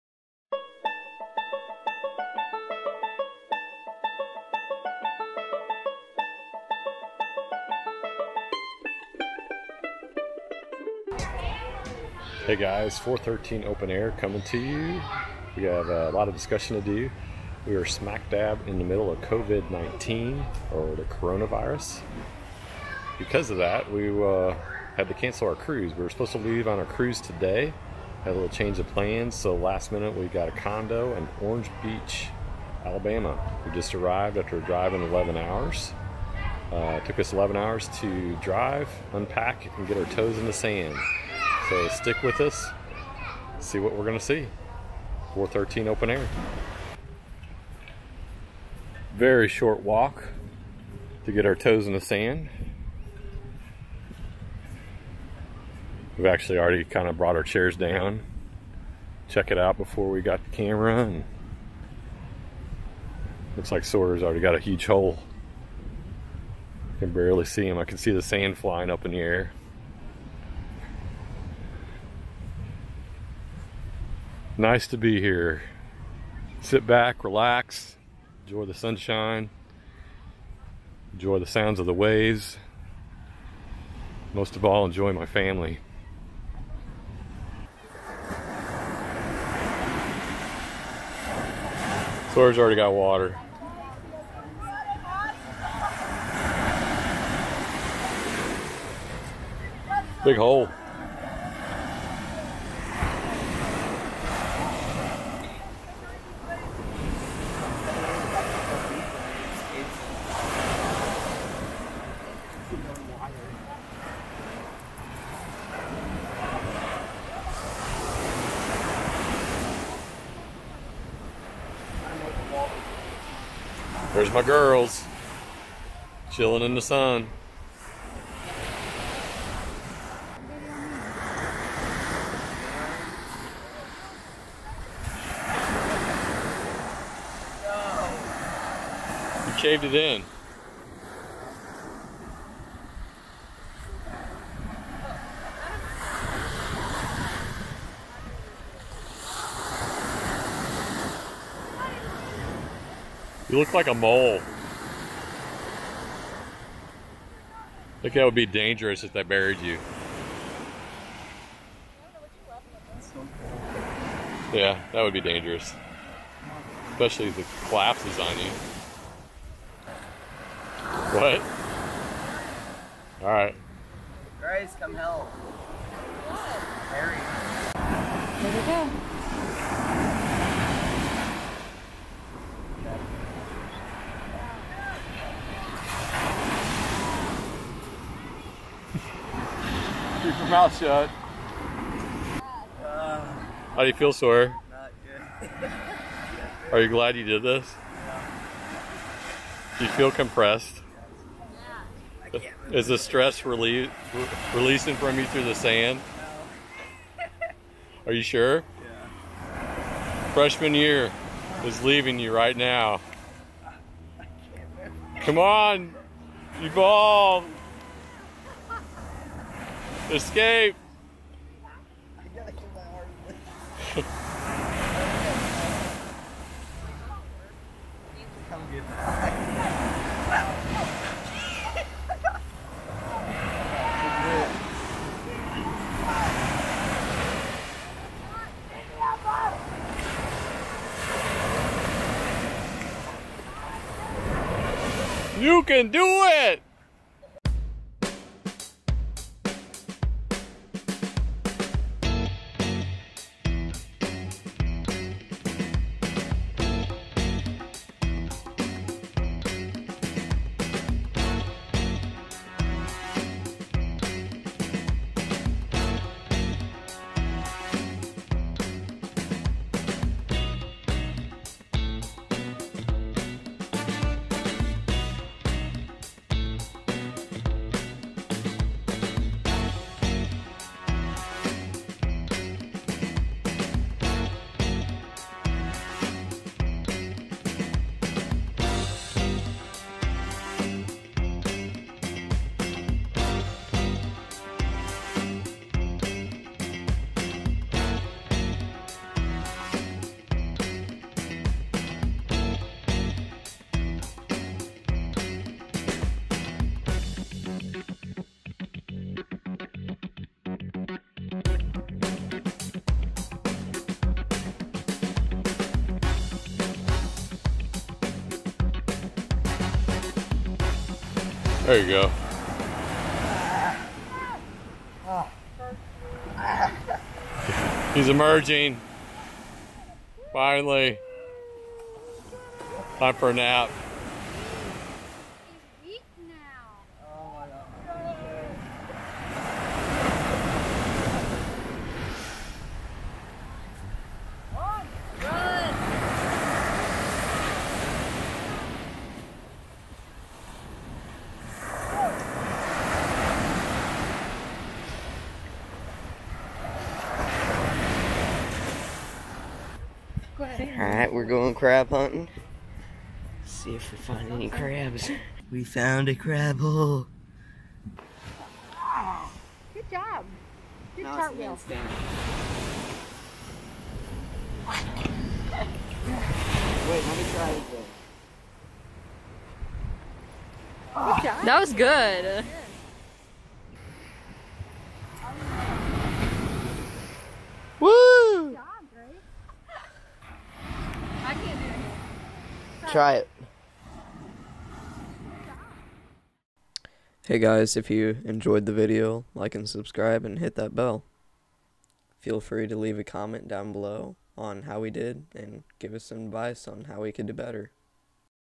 COVID19 Cancels Cruise, but Plan B is Awesome!
The escalating concerns of the COVID19 virus caused the cancellation of our spring break cruise with our family.  We then found a different way for some sunshine and sand!  Next stop Orange Beach Alabama.  This is our first time on this beach getaway.  It was nice to have a ground floor condo with steps away from the beach.